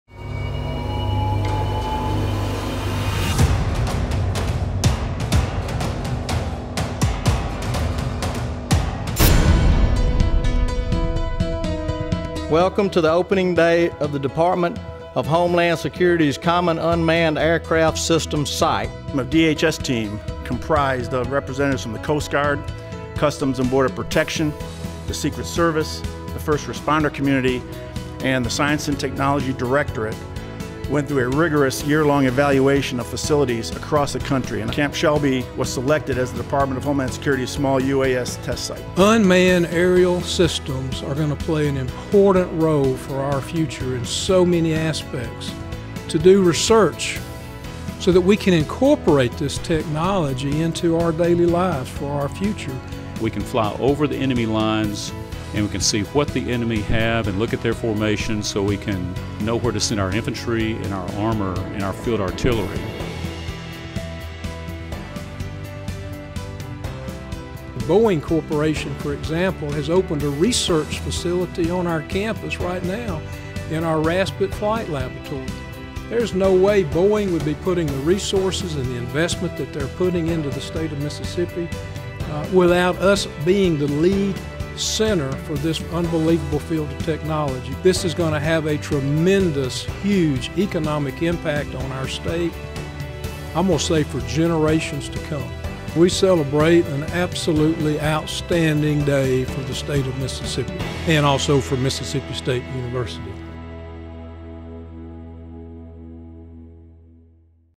MSU launches major research project for Homeland Security
New partnership to impact state for generations to come